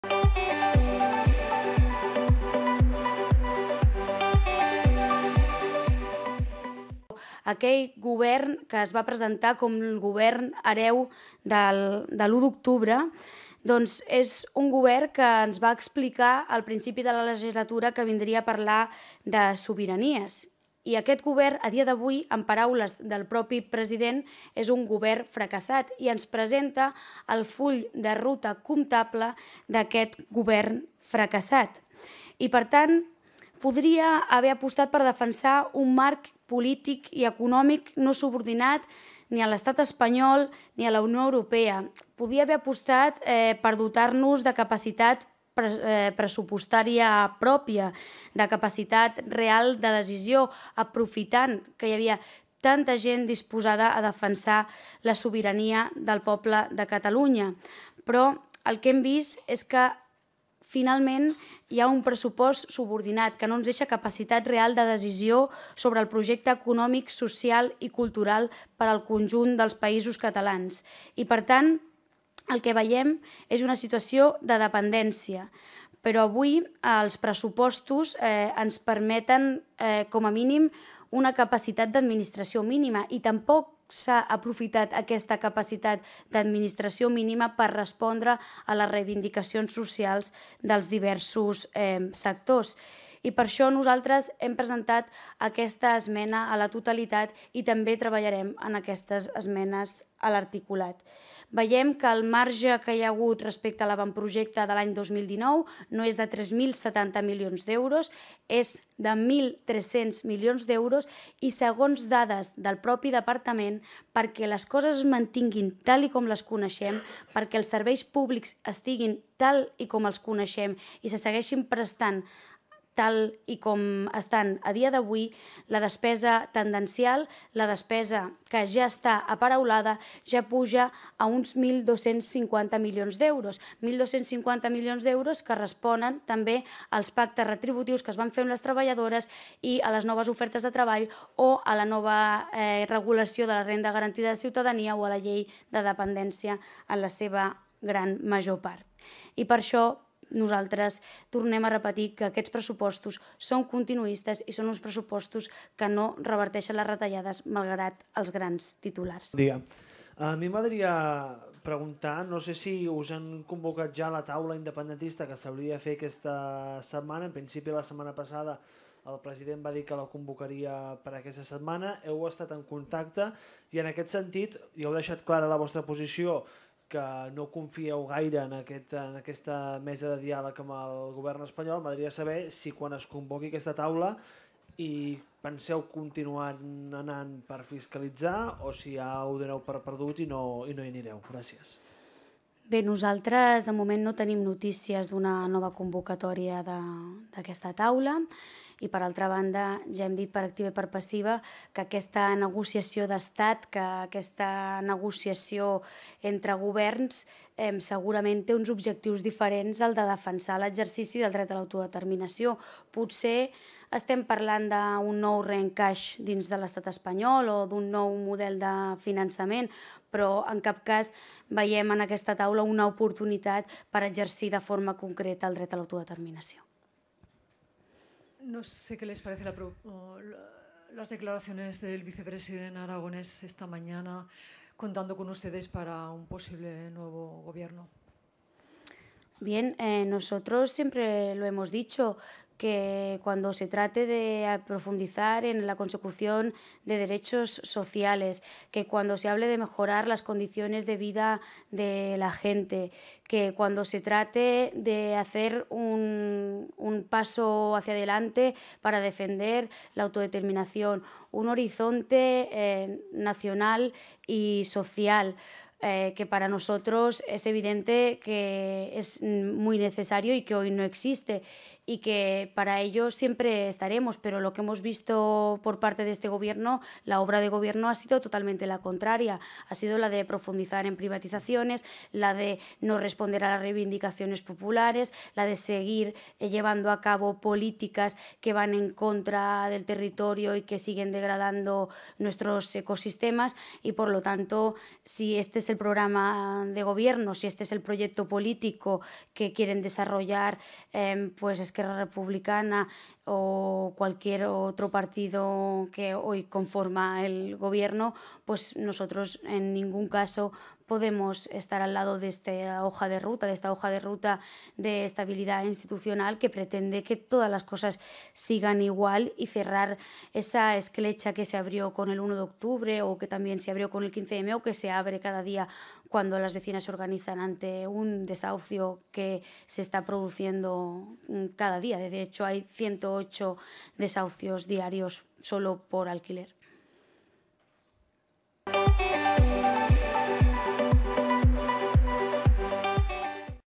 Maria Sirvent (CUP) descarta entrar en un Govern con una Hoja de Ruta de estabilidad institucional
Rueda de prensa en el Parlament de Catalunya 11 de Febrero de 2020 de Maria Sirvent CUP descarta entrar en un Govern con una Hoja de Ruta de estabilidad institucional. Fuente Canal Parlament.